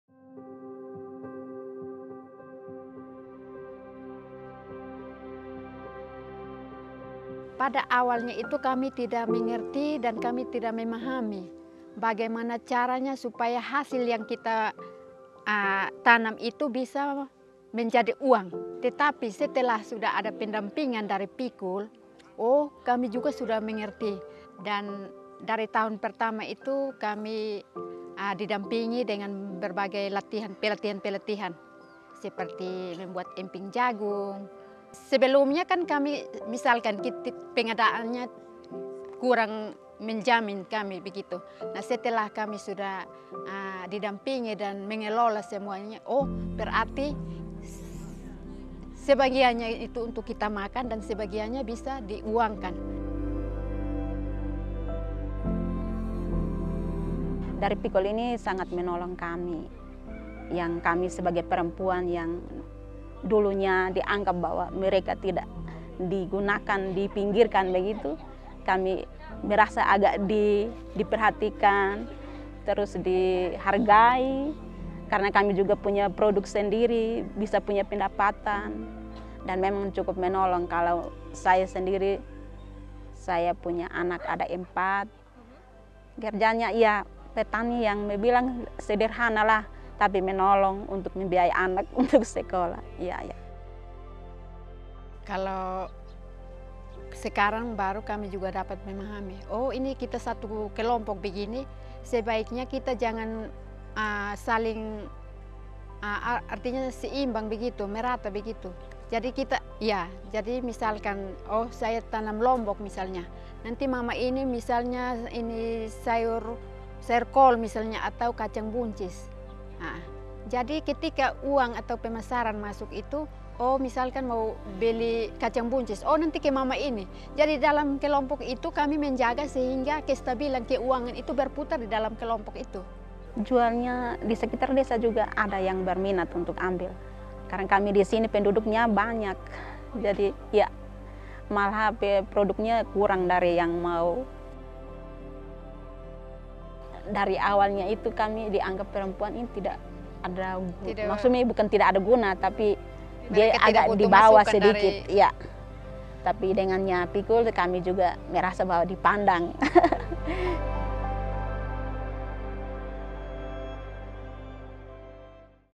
Climate Resilient Sustainable Livelihoods in Indonesia
As part of the Indonesia Disaster and Climate Resilient Communities Project (ICDRC), Oxfam local partner, PIKUL, is training and mentoring women like Uri and Damaris. Uri and Damaris are farmers, who are building climate resilient sustainable livelihoods after training with PIKUL. The training helped to increase their farming yields and to sell any excess produce for extra income.

Oxfam acknowledges the support of the Australian Government through the Australian NGO Cooperation Program (ANCP).